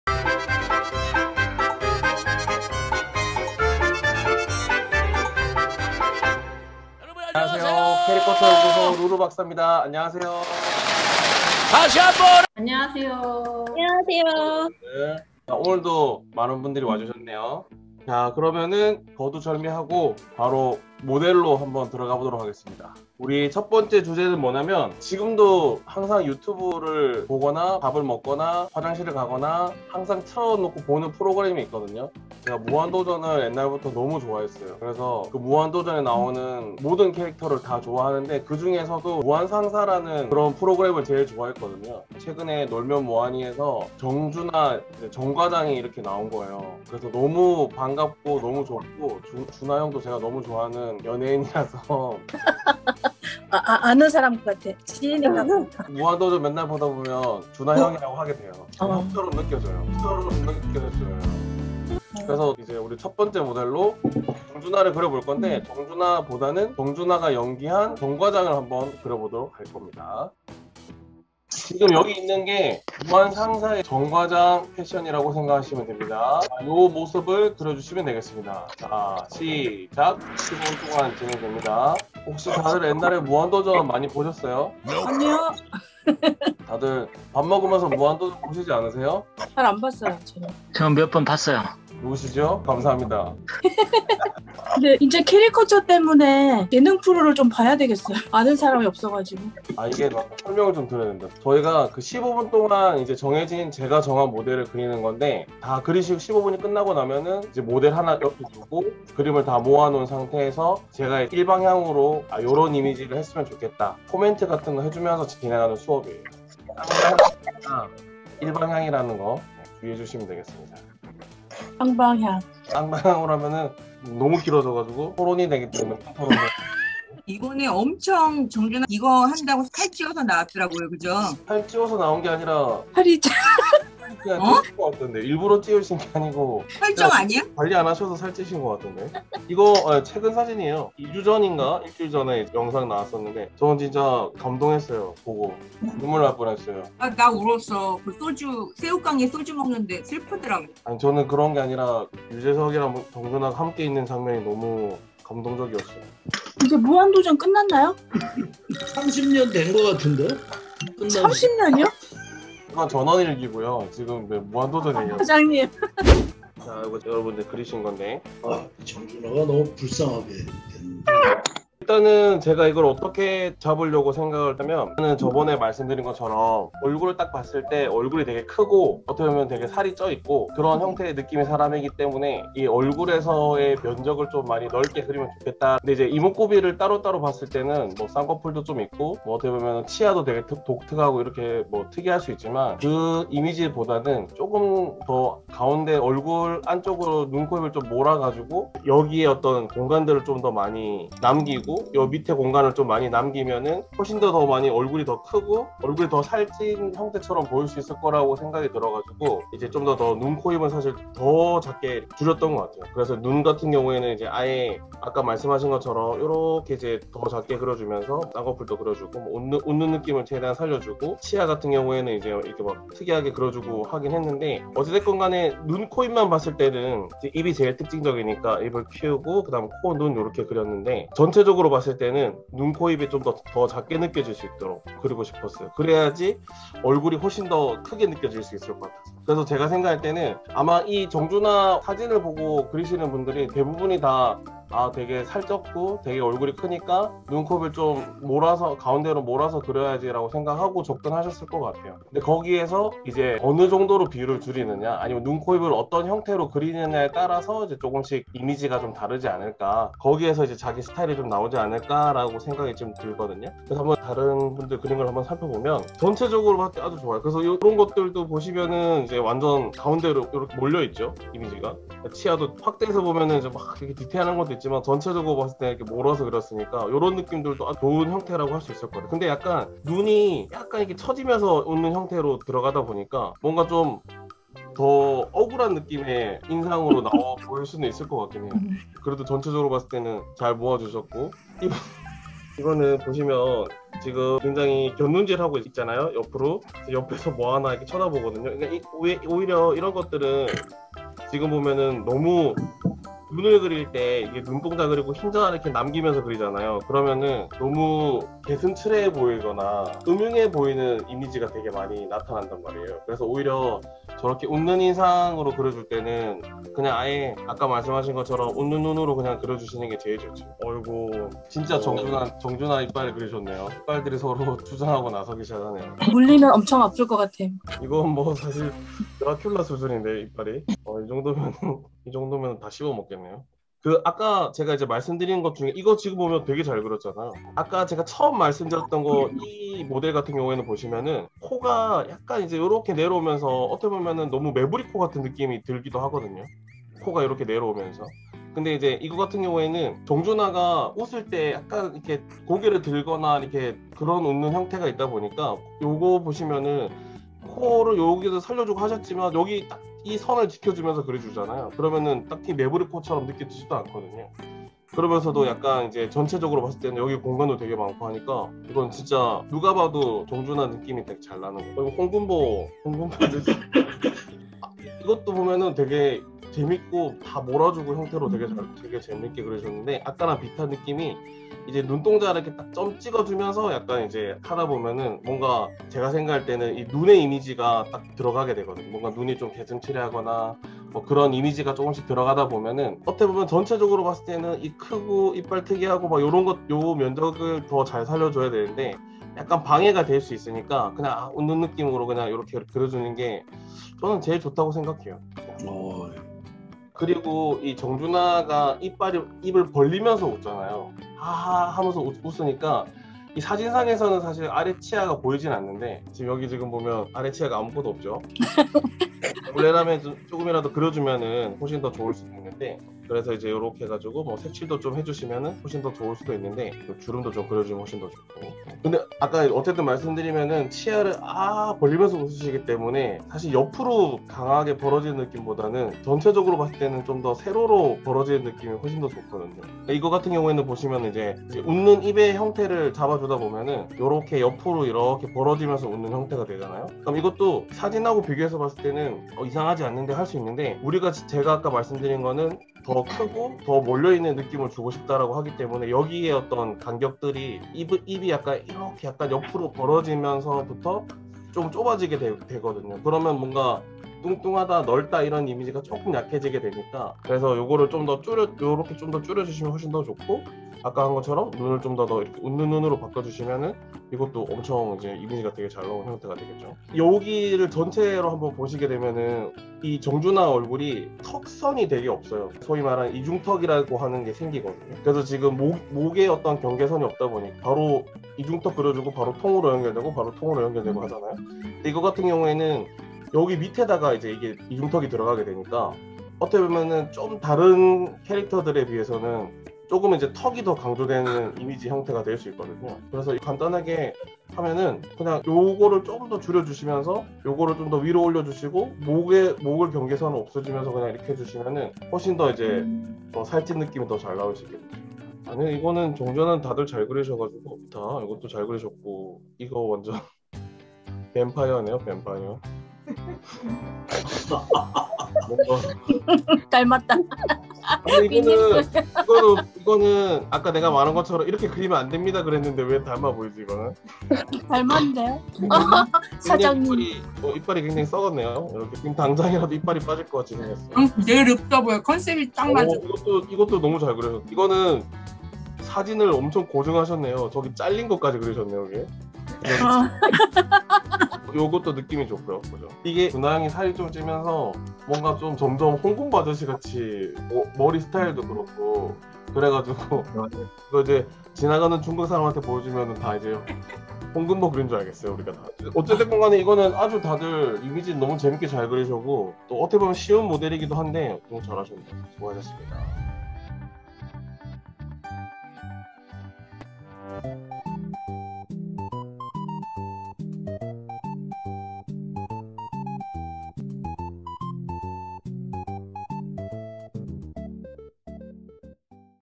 "야~무지게 그려야징~!" _ [정준하 캐리커쳐] 그려보기
온라인으로 진행되는 소소한 캐리커처 스터디

캐리커처연구소 루루박사!
Episode 06 - (1) '정준하' 편 06.18

* 사연 및 사진 보내주실 곳
성함, 사진(1-2매), 간략한 사연을 적어서 보내주시면
다음 회차 연구소에서 작가님들과 드로잉 해드립니다 :)